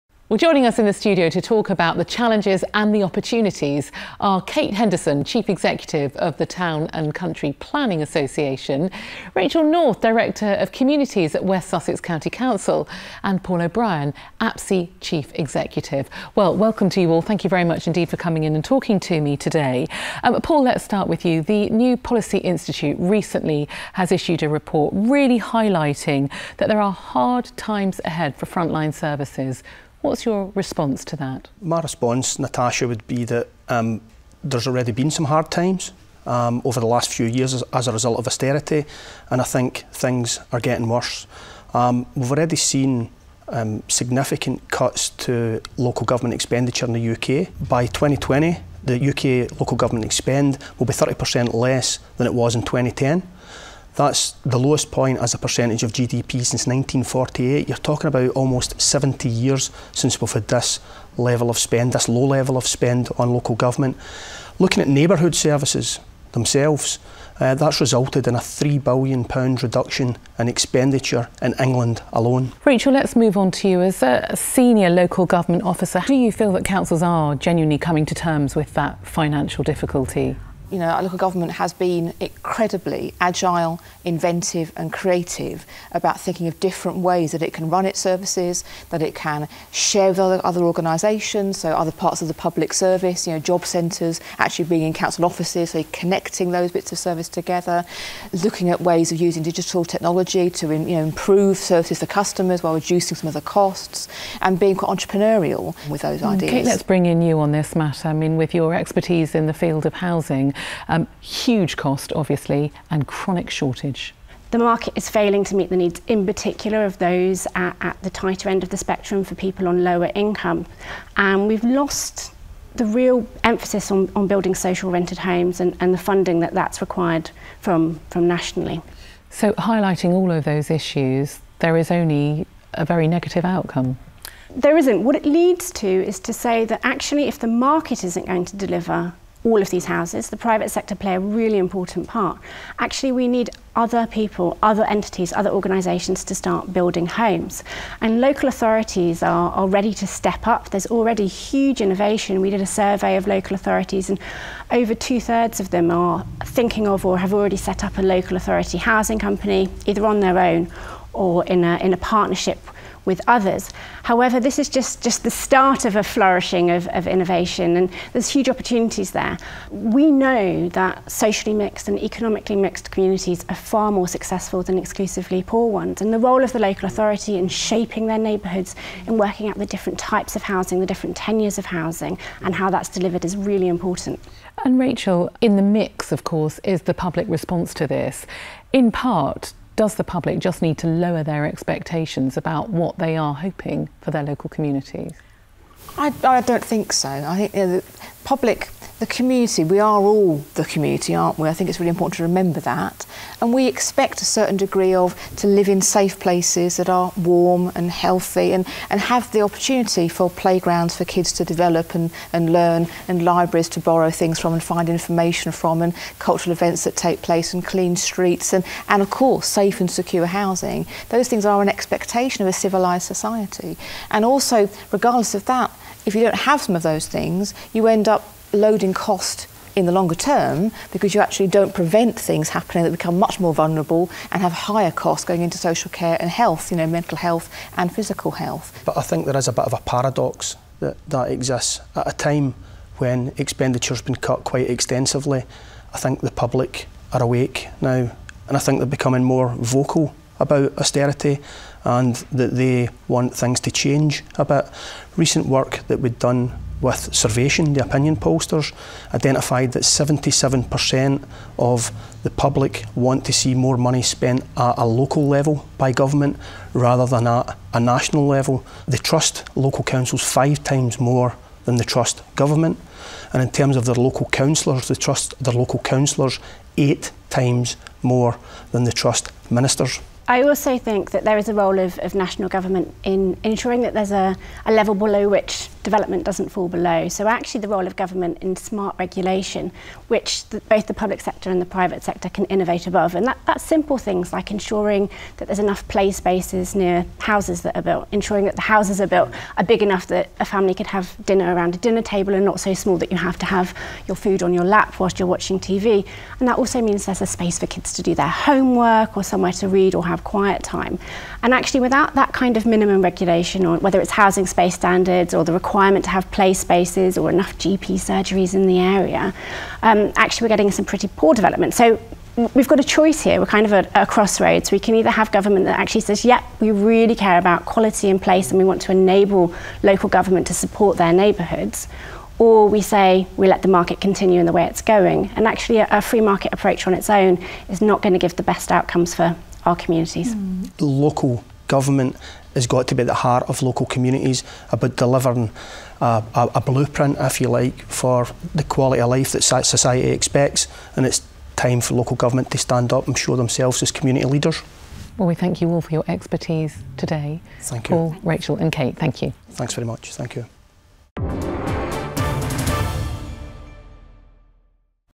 Challenges and Opportunities Faced in Our Neighbourhoods – ITN Productions Studio Debate
Studio debate with APSE and Natasha Kaplinsky